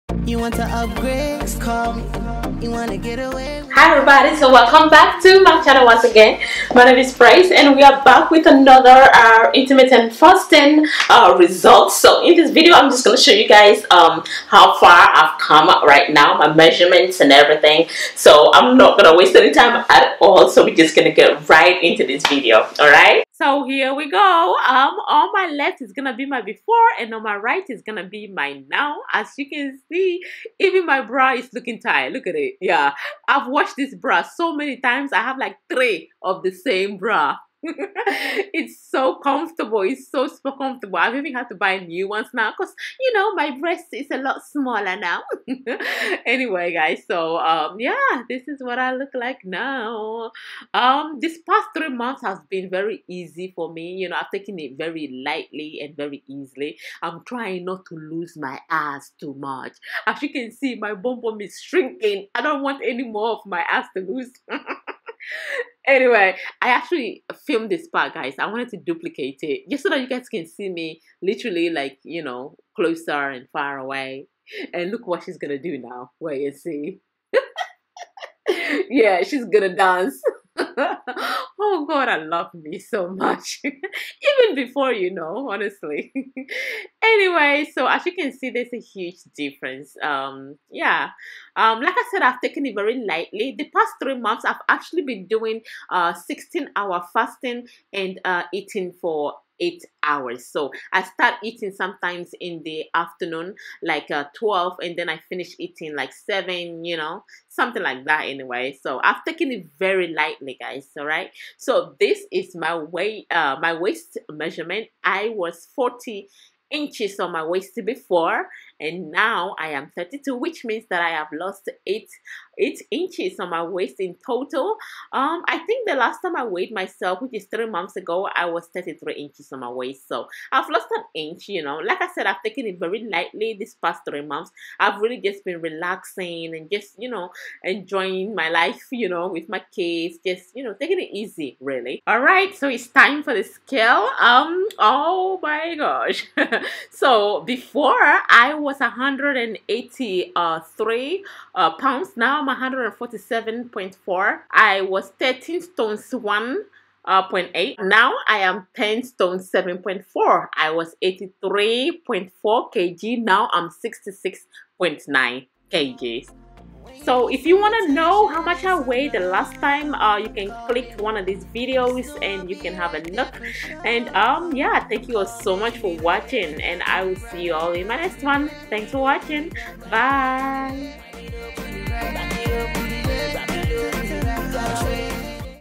Intermittent Fasting Before & After Result Fat Burning Weight Loss Transformation
So Far Intermittent Fasting is the best way for me to burn fat and lose weight quick and effectively. In this video i show you my results before and after, my weight loss and measurement.